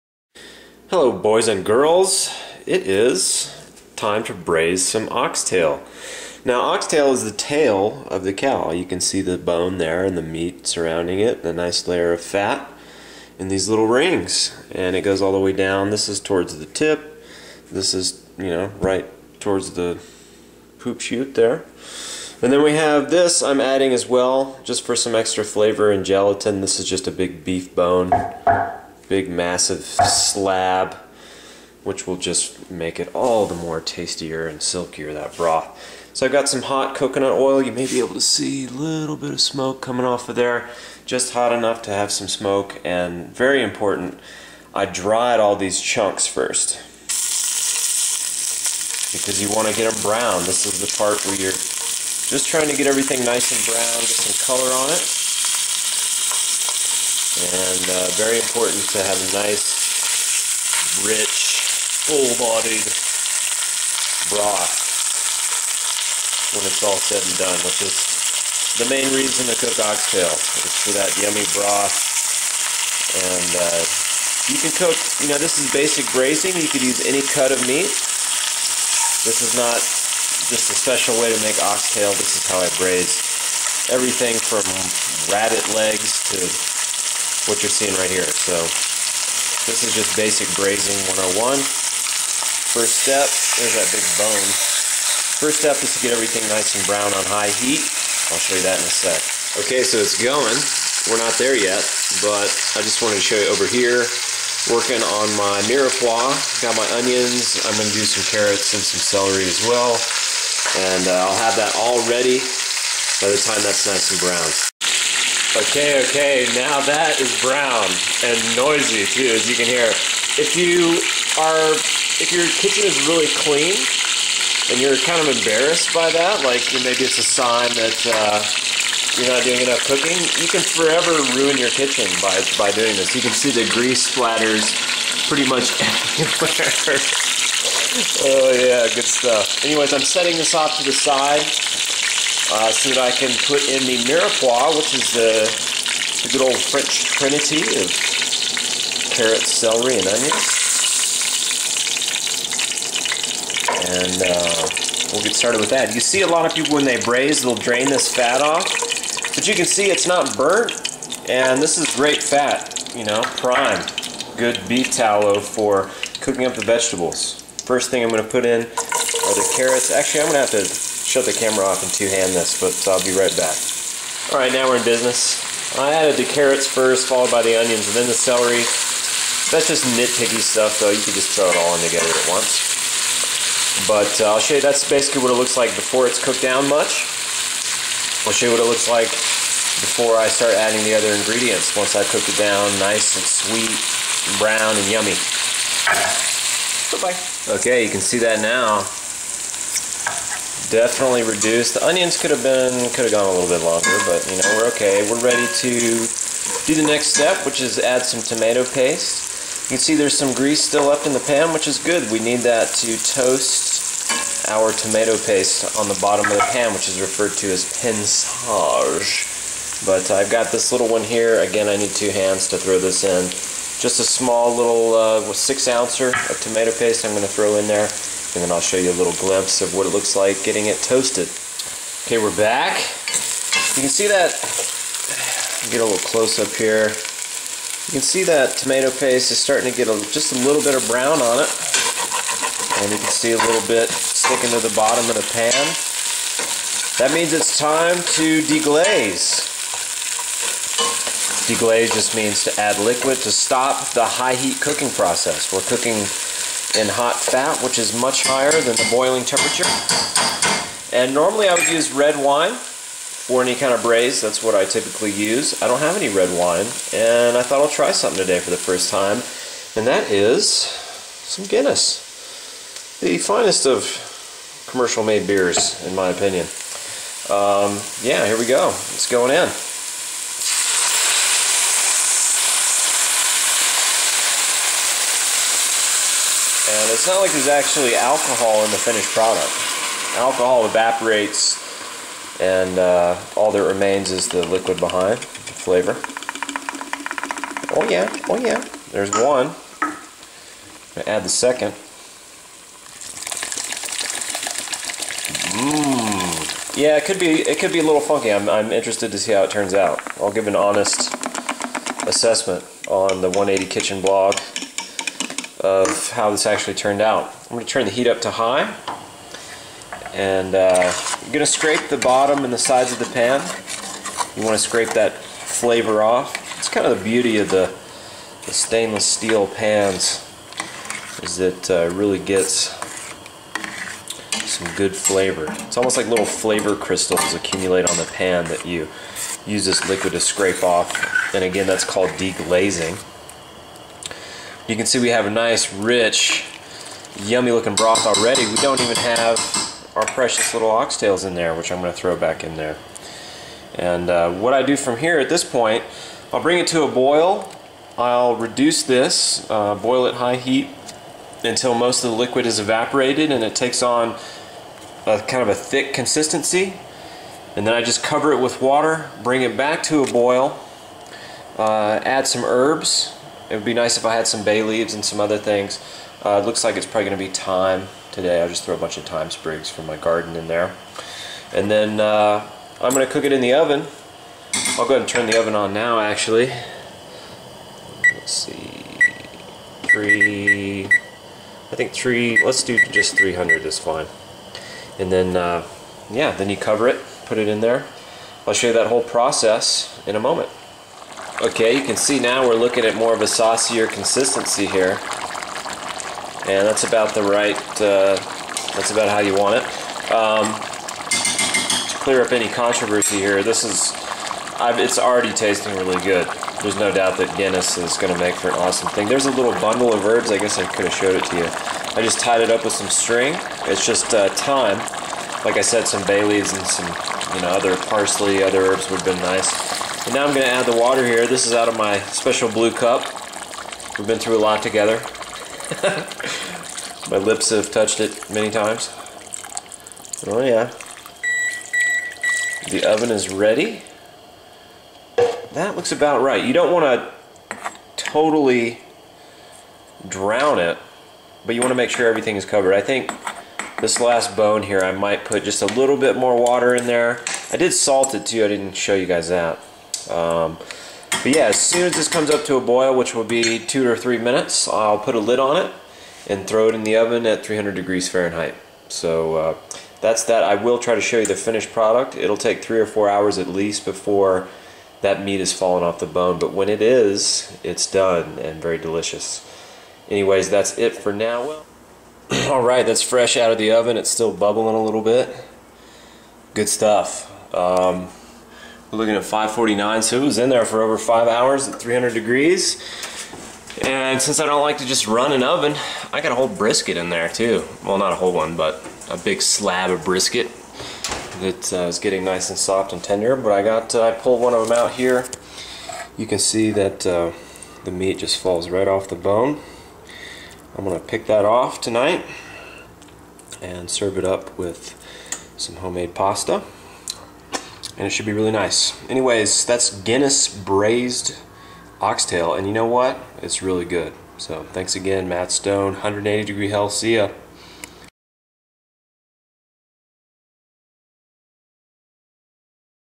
How to Braise Oxtail, Short Ribs, and Other Meats
Braising is a great cooking method as we enter winter.  Oxtail stew, braised short ribs, braised rabbit, braised lamb shanks - all are done using this basic and true form of French braising technique.  Usually I do a red wine braise, but this time I made Guiness-braised oxtail stew.  Um, holy cow!!!